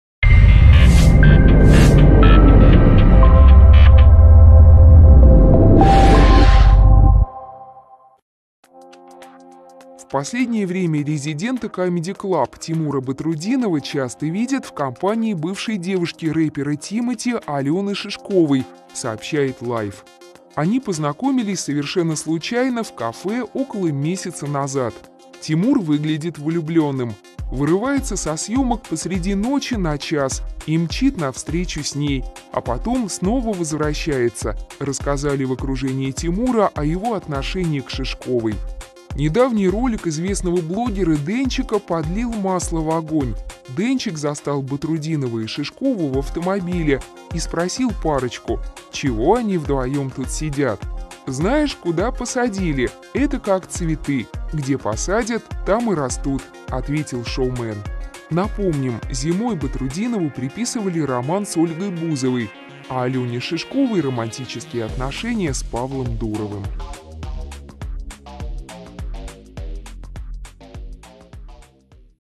Тимура Батрутдинова застукали в машине с Аленой Шишковой
Батрутдинова заметили в компании бывшей девушки Тимати